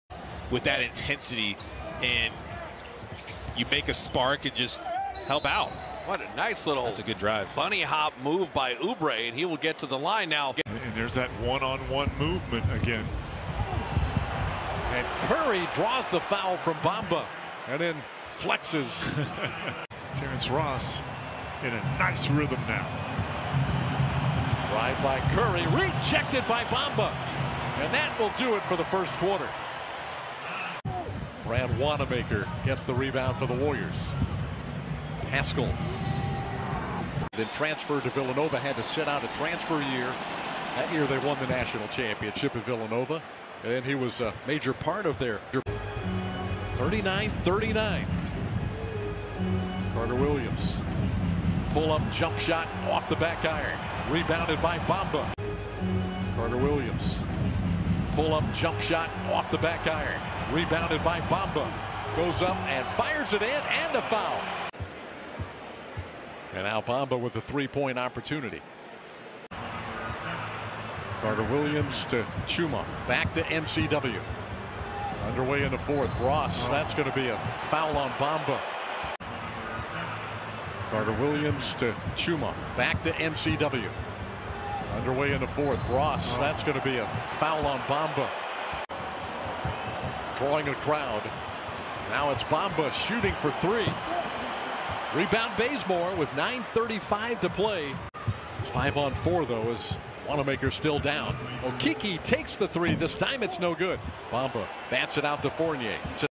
Mo Bamba : All Possessions (2021-02-20)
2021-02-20 Orlando Magic vs Golden State Warriors: Player Highlights Mo Bamba
    3 PTS, 3 REB, 0 AST, 0 STL.
        0 of 1 from 3.
        1 of 2 FGs in PT09M02.00S minutes.